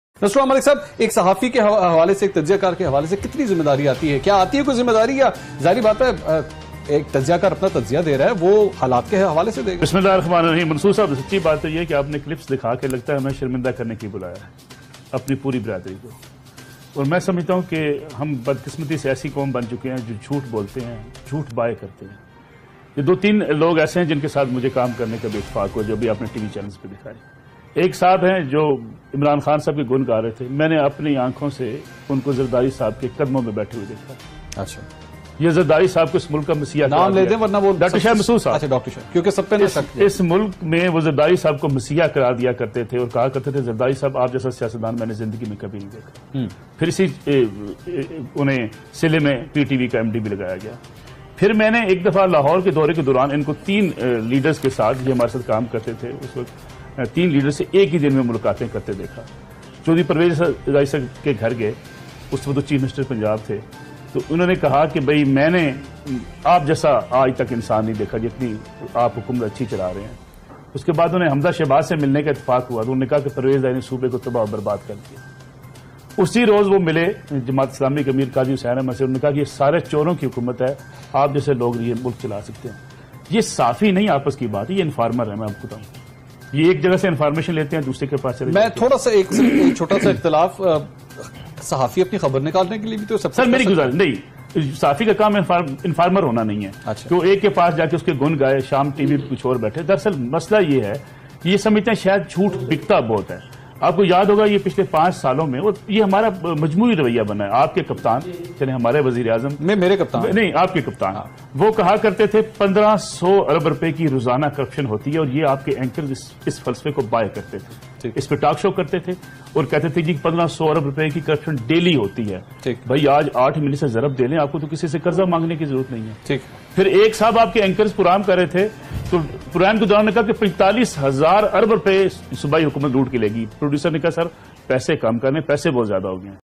Dr. Shahid Masood Ko Apni Aankhon Se Zardari Ke Qadamon Mein Baithe Daikha : Nasrullah Malik
In this Video Famous Journalist Exposed the Reality of Dr. Shahid Masood Ko Apni Aankhon Se Zardari Ke Qadamon Mein Baithe Daikha - Nasrullah Malik